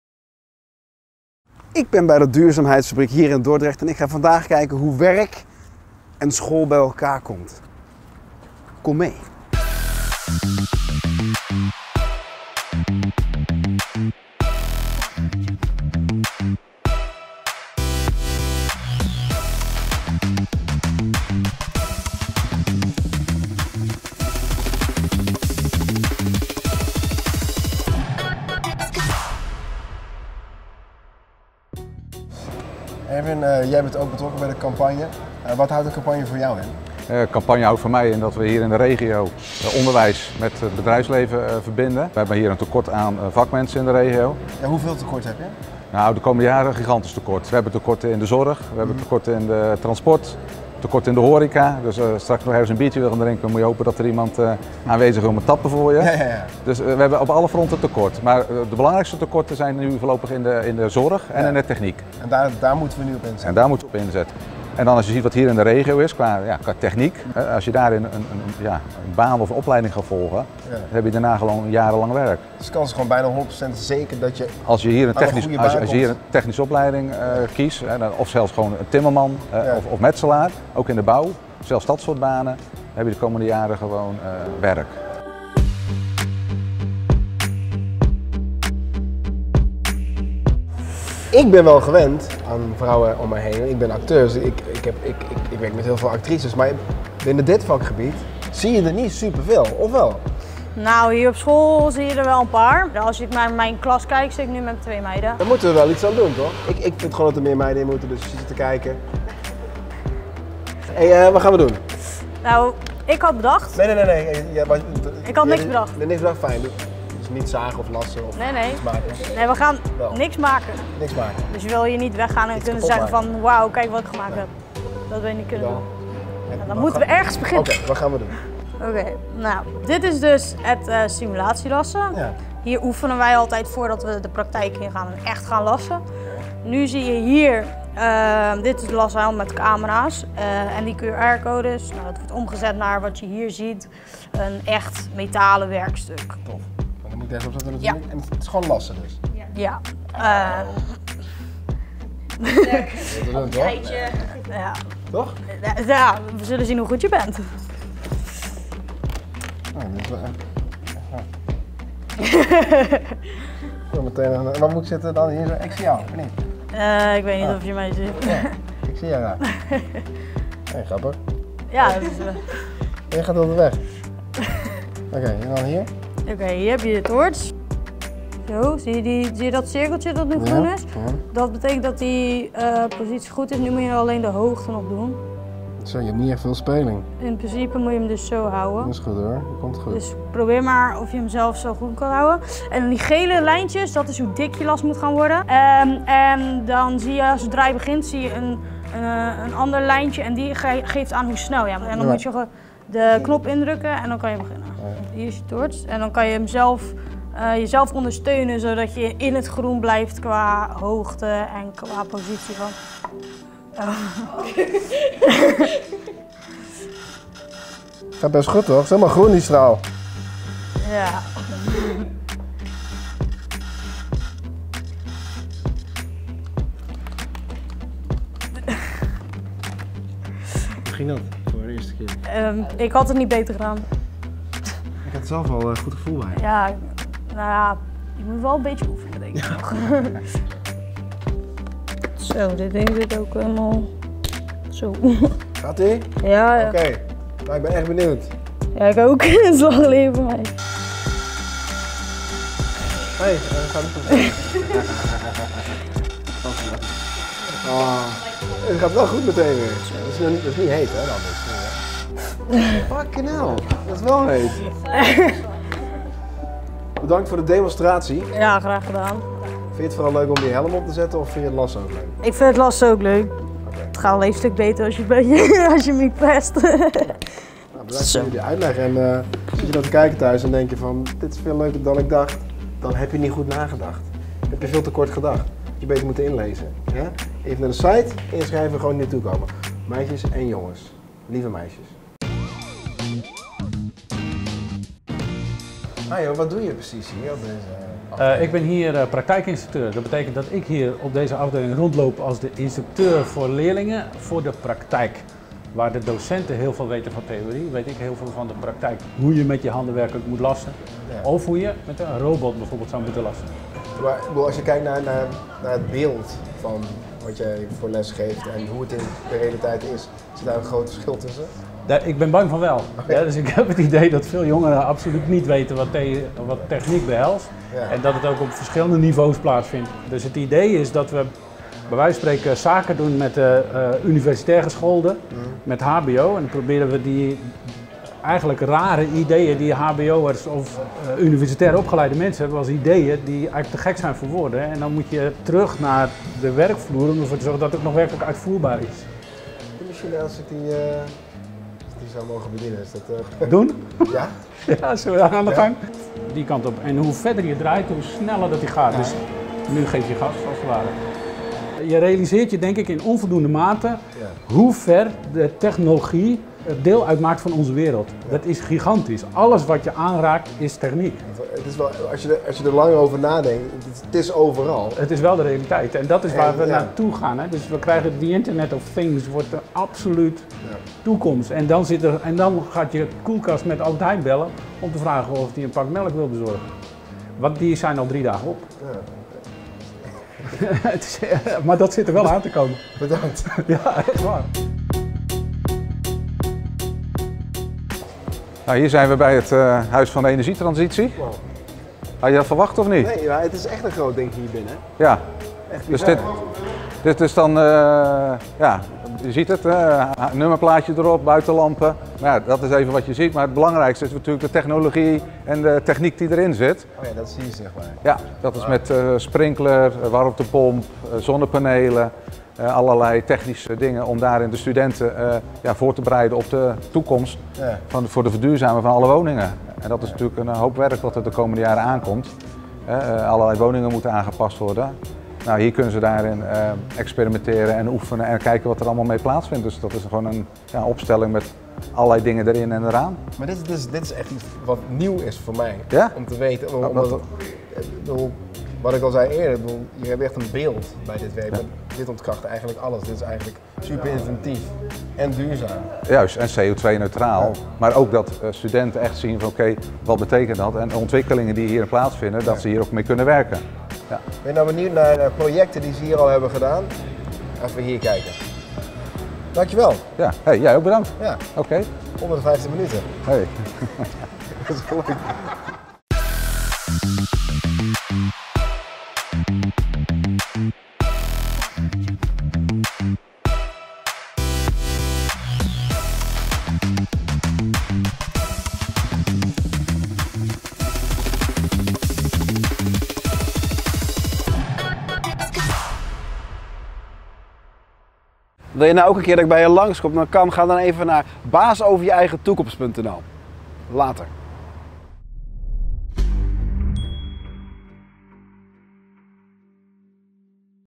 Baas Over Je Eigen Toekomst | Introductie Tim Haars - Duurzaamheidsfabriek Dordrecht
Tim Haars presenteert het nieuwe online programma "Baas over je eigen toekomst" maar wat is dat dan precies.

Hij gaat met mensen hierover in gesprek bij de Duurzaamheids- Fabriek te Dordrecht om hier over te babbelen en te ervaren waarom dit programma zo belangrijk is.

‘Baas over je eigen toekomst’ is een samenwerking van gemeenten, werkgevers, brancheorganisaties en opleiders in de Drechtsteden.

De komende jaren zorgt de economische groei voor duizenden extra mbo-banen in de regio. Om daar op een goede manier invulling aan te kunnen geven, hebben we jou hard nodig! Iedereen is baas over zijn of haar eigen toekomst. Neem jij daarom vandaag nog het heft in handen om die droombaan of opleiding te grijpen? Wacht niet langer en zet de eerste stap. Wij helpen je graag op weg.

Meer info? Ga dan naar baasoverjeeigentoekomst.nl